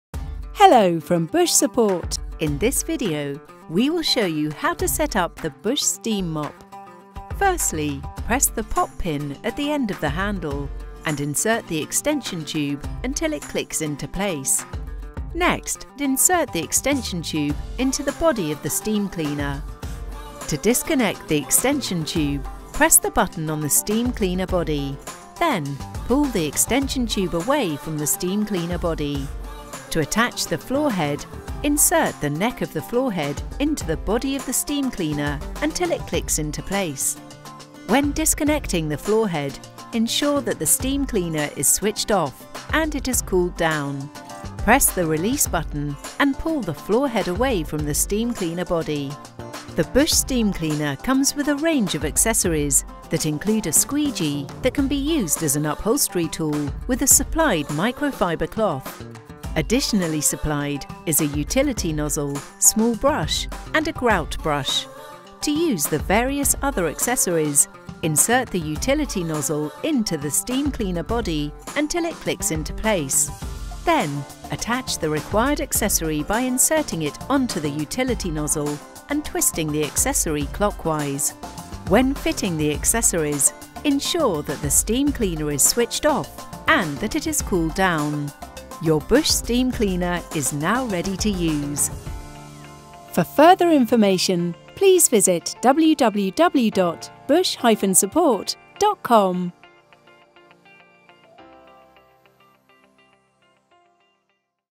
How to setup the Bush Upright Steam Mop
In this Bush Support video we will show you how to setup the Bush Upright Steam Mop.